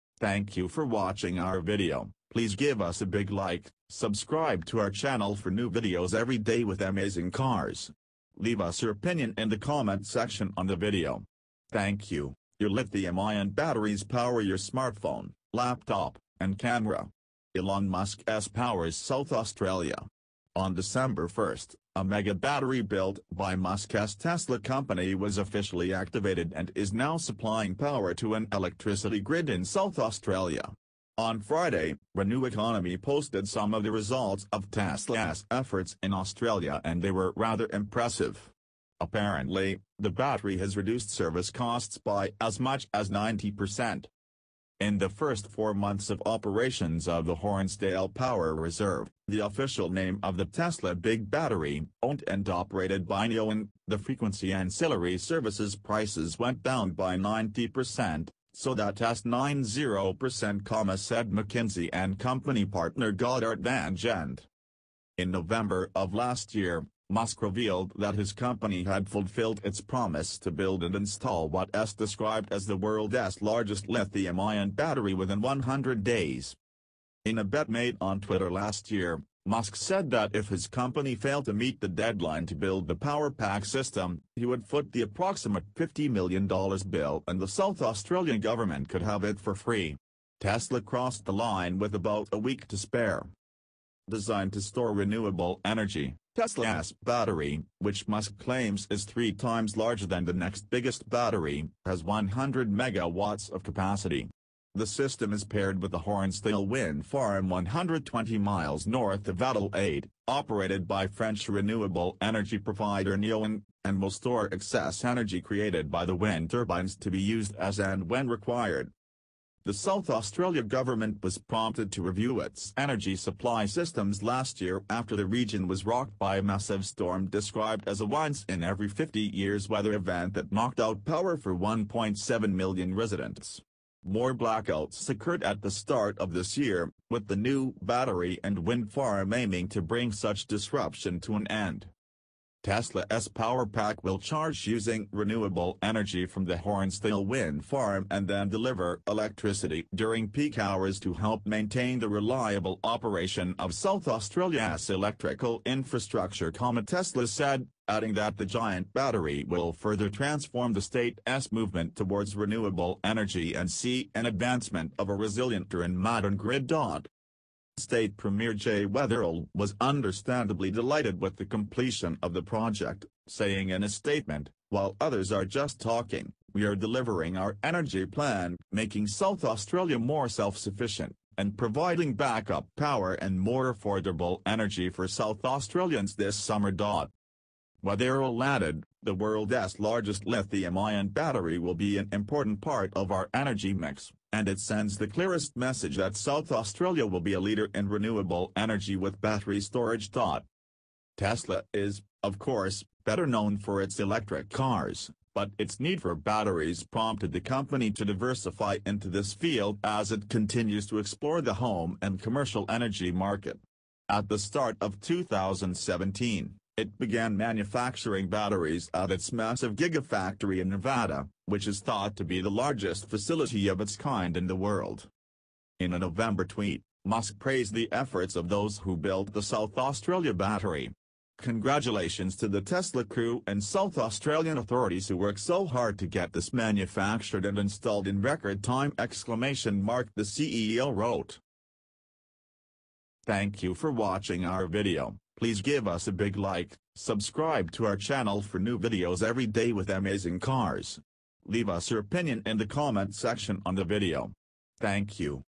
Tesla’s giant Australian battery reduced service costs by 90 percent
Your lithium-ion batteries power your smartphone, laptop, and camera. Elon Musk’s powers South Australia. On December 1, a mega-battery built by Musk’s Tesla company was officially activated and is now supplying power to an electricity grid in South Australia. On Friday, RenewEconomy posted some of the results of Tesla’s efforts in Australia and they were rather impressive. Apparently, the battery has reduced service costs by as much as 90 percent.

“In the first four months of operations of the Hornsdale Power Reserve (the official name of the Tesla big battery, owned and operated by Neoen), the frequency ancillary services prices went down by 90 per cent, so that’s 9-0 per cent,” said McKinsey and Co. partner Godart van Gendt.

In November of last year, Musk revealed that his company had fulfilled its promise to build and install what’s described as “the world’s largest lithium-ion battery” within 100 days.

In a bet made on Twitter last year, Musk said that if his company failed to meet the deadline to build the Powerpack system, he would foot the approximate $50 million bill and the South Australia government could have it for free. Tesla crossed the line with about a week to spare.

Designed to store renewable energy, Tesla’s battery, which Musk claims is three times larger than the next biggest battery, has 100 megawatts of capacity. The system is paired with the Hornsdale wind farm 120 miles north of Adelaide, operated by French renewable energy provider Neoen, and will store excess energy created by the wind turbines to be used as and when required.

The South Australia government was prompted to review its energy supply systems last year after the region was rocked by a massive storm described as a once-in-every-50-years weather event that knocked out power for 1.7 million residents. More blackouts occurred at the start of this year, with the new battery and wind farm aiming to bring such disruption to an end.

“Tesla’s Powerpack will charge using renewable energy from the Hornsdale Wind Farm and then deliver electricity during peak hours to help maintain the reliable operation of South Australia’s electrical infrastructure,” Tesla said, adding that the giant battery “will further transform the state’s movement towards renewable energy and see an advancement of a resilient and modern grid.”

State Premier Jay Weatherill was understandably delighted with the completion of the project, saying in a statement, “While others are just talking, we are delivering our energy plan, making South Australia more self-sufficient, and providing back up power and more affordable energy for South Australians this summer.”

Weatherill added, “The world’s largest lithium-ion battery will be an important part of our energy mix, and it sends the clearest message that South Australia will be a leader in renewable energy with battery storage.”

Tesla is, of course, better known for its electric cars, but its need for batteries prompted the company to diversify into this field as it continues to explore the home and commercial energy market. At the start of 2017, it began manufacturing batteries at its massive Gigafactory in Nevada, which is thought to be the largest facility of its kind in the world.

In a November tweet, Musk praised the efforts of those who built the South Australia battery. “Congratulations to the Tesla crew and South Australian authorities who worked so hard to get this manufactured and installed in record time!” the CEO wrote.